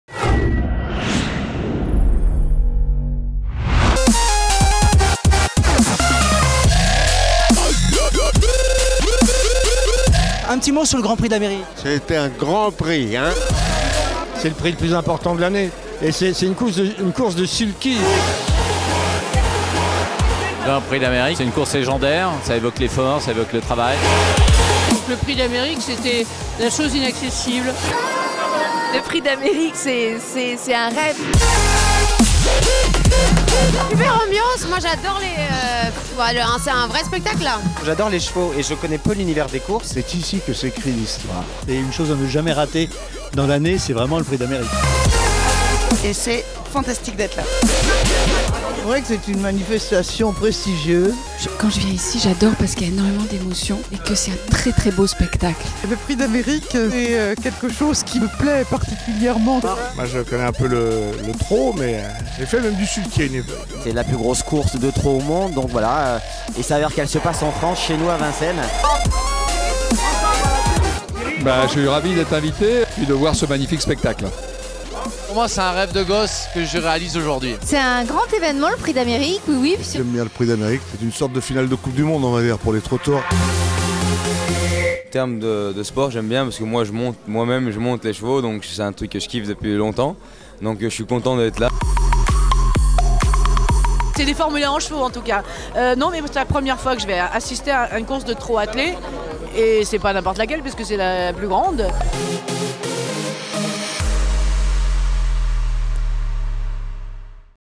Grand Prix d'Amérique, un rendez-vous ultra VIP !
Le Grand Prix d'Amérique c'est le Championnat du Monde du TROT attelé où chaque année se presse le tout Paris.